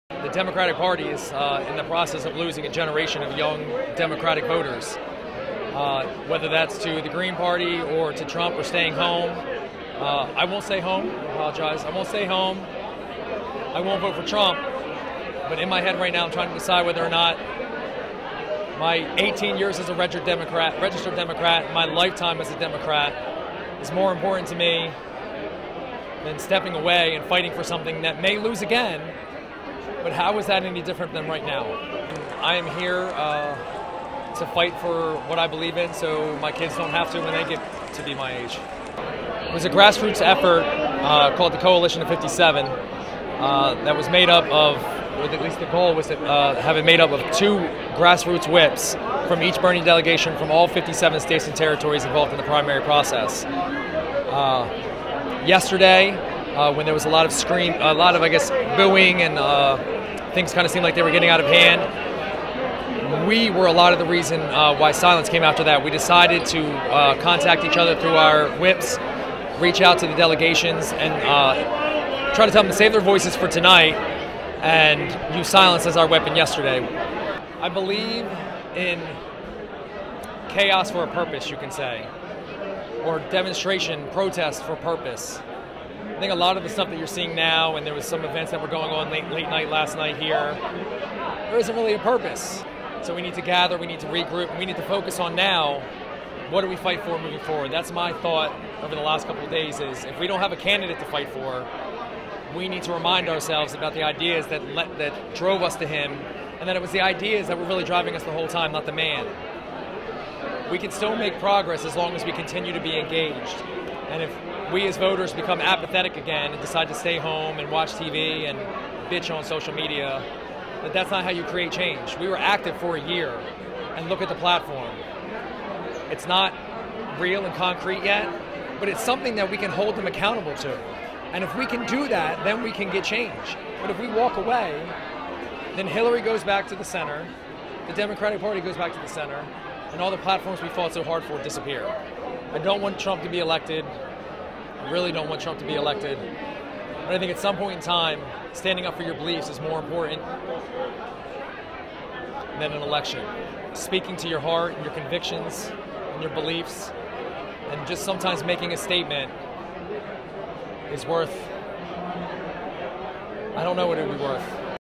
Del. Sanders delegate: "Democratic party losing a generation of voters"
Delaware Bernie Sanders delegate Jeff Day is conflicted on how he will proceed now that Hillary Clinton is the democratic nominee for the President of the United States.